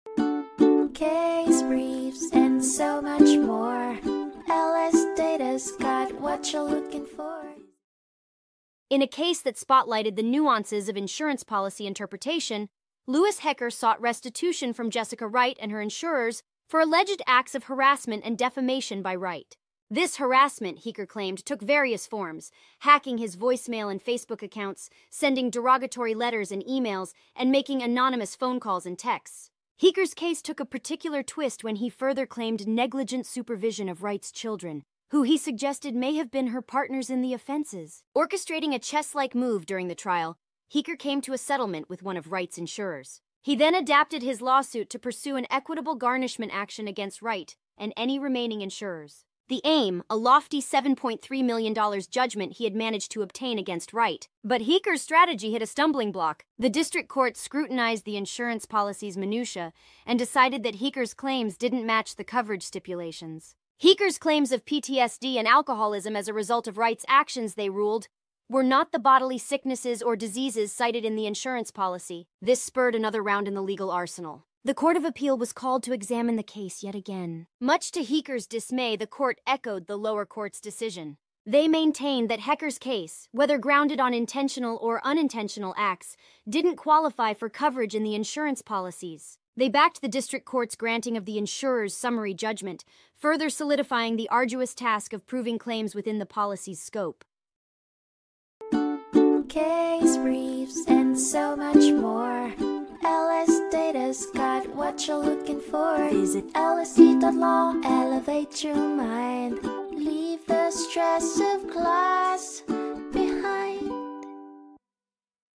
Heacker v. Safeco Insurance Co. of America (2012) Overview | LSData Case Brief Video Summary
Lewis Heacker sued Jessica Wright and her insurers because he claimed that she harassed and defamed him by hacking into his voicemail and Facebook, sending disparaging letters and emails, and making anonymous phone calls and texts. He settled with one of her insurers during the trial and then sued Wright and her remaining insurers for an equitable garnishment action to collect a $7.3 million judgment he obtained against her. The district court held that his claims did not fall under the coverage provided by the insurance policies.

Heacker v. Safeco Insurance Co. of America (2012)
United States Court of Appeals for the Eighth Circuit
676 F.3d 724